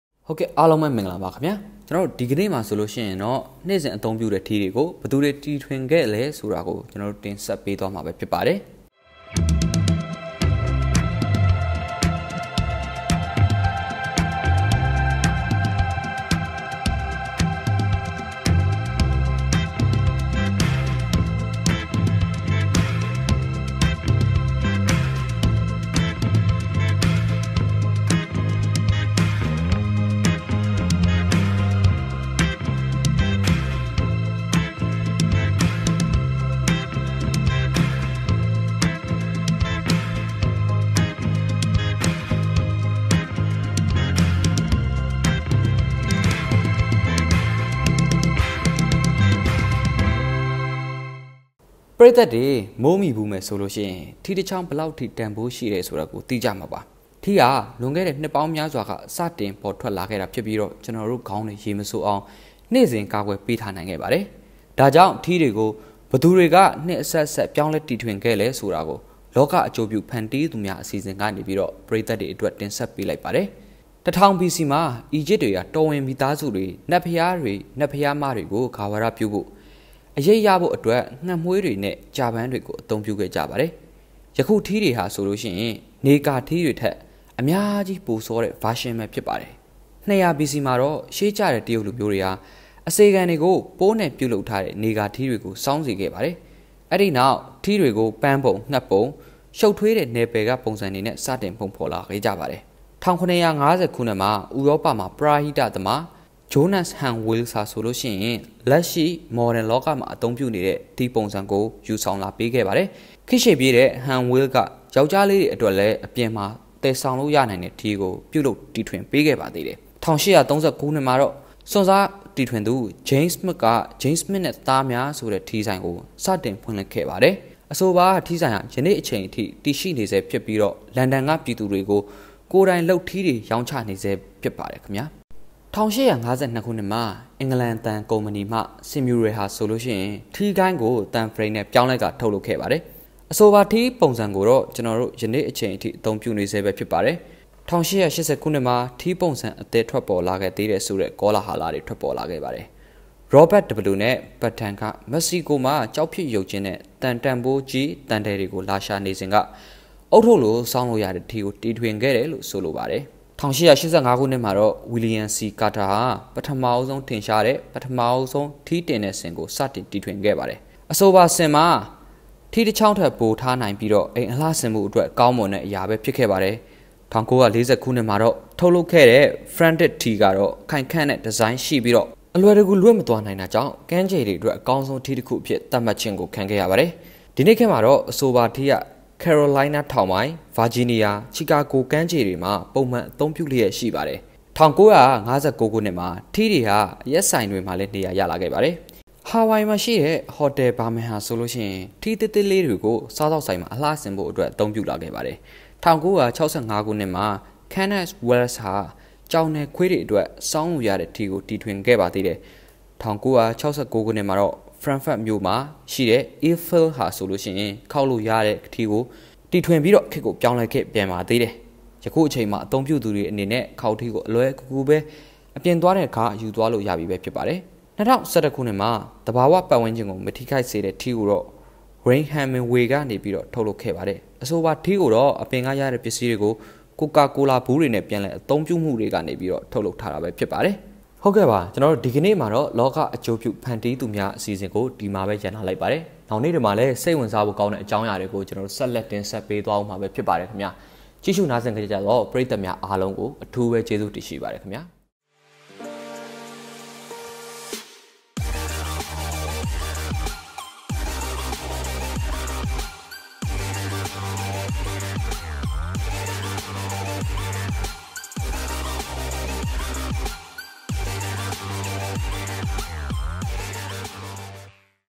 နေ့စဉ်အသုံးပြုနေတဲ့ ထီးတွေကို ဘယ်သူတီထွင်ခဲ့တာလဲ
ပရိသတ်တွေ မိုးမိဖူးမယ်ဆိုရင် ထီးတစ်ချောင်း ဘယ်လောက်ထိ တန်ဖိုးရှိမယ်ဆိုတာ သိကြမှာပါ။
ထီးဟာ လွန်ခဲ့တဲ့ နှစ်ထောင်ပေါင်းများစွာက စတင်ပေါ်ထွက်လာပြီးနောက် ကျွန်တော်တို့ ခေါင်းတွေ ရေမစိုအောင် နေ့စဉ် ကာကွယ်ပေးထားနိုင်ခဲ့ပါတယ်။
ဒါကြောင့် ထီးတွေကို ဘယ်သူတွေ နှစ်အဆက်အဆက် ပြောင်းလဲတီထွင်ခဲ့လဲဆိုတာ လောကအကျိုးပြု ဖန်တီးသူများ အစီအစဉ်ကနေ  ပရိသတ်တွေအတွက် တင်ဆက်ပေးလိုက်ပါတယ်။